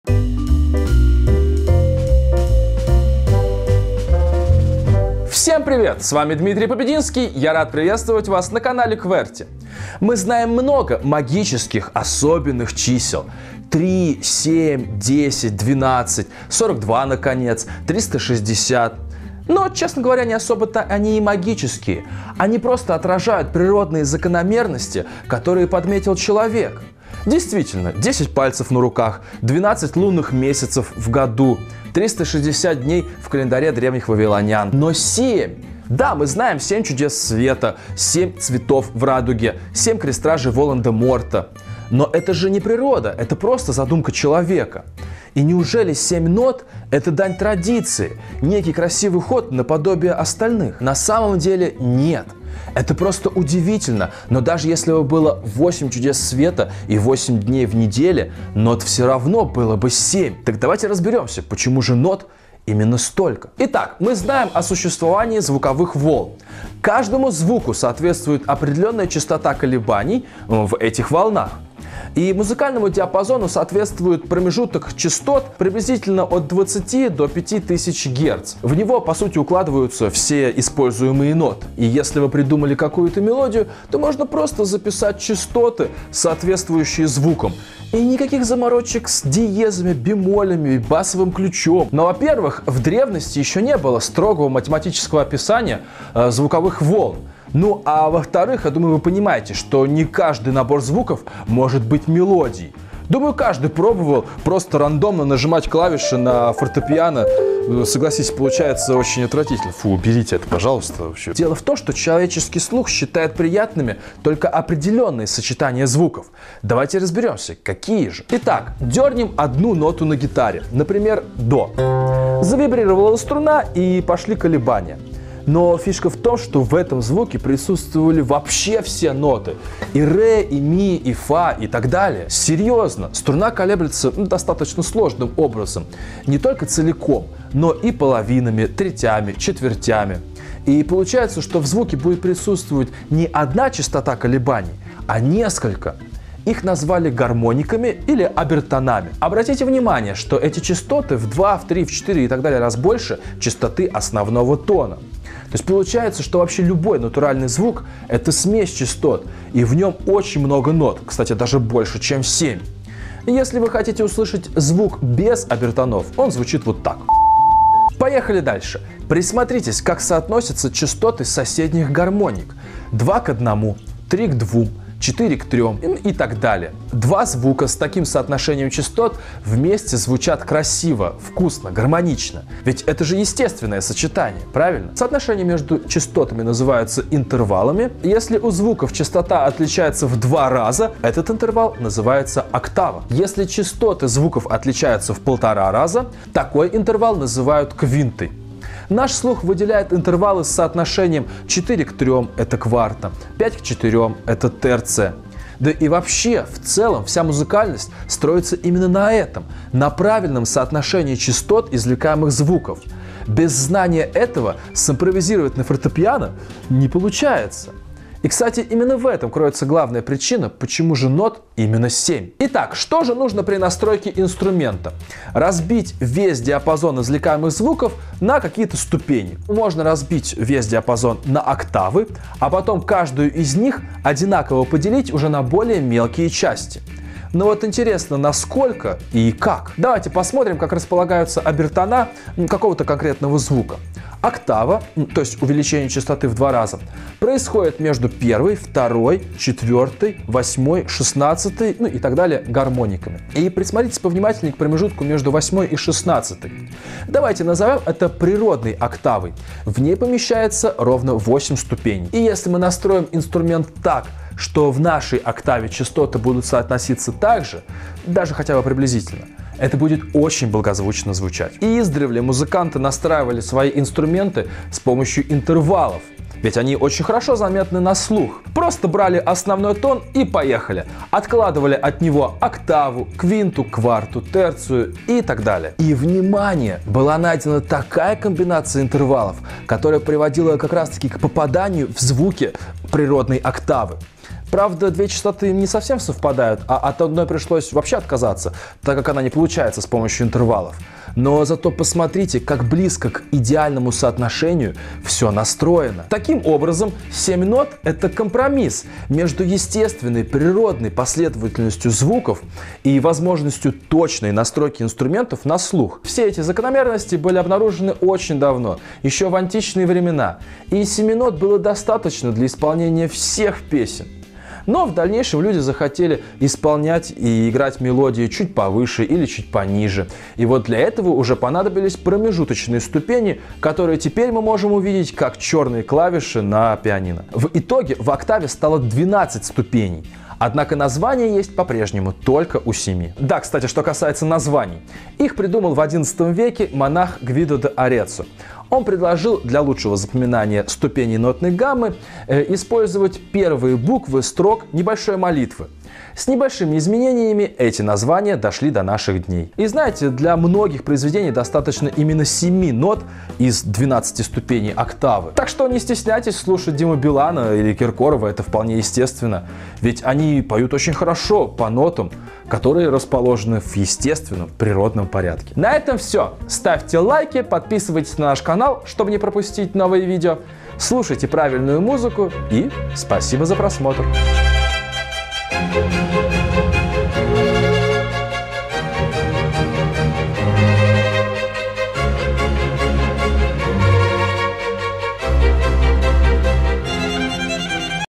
Почему нот семь?
7 чудес света, 7 дней в неделе, 7 цветов в радуге. Кажется эта магическая цифра 7 всюду нас преследует!
Но чем определяется количество нот? Почему их именно 7? Что это - просто случайность или закономерность, продиктованная законами физики?